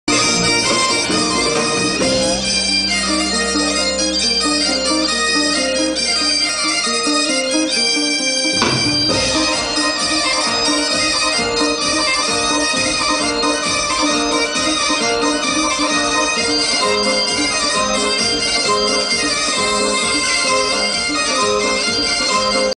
Yaouank 2007
4e représentation du Spectacle War'l Leur au Fest-Noz "Yaouank" à Rennes le 17 novembre 2007.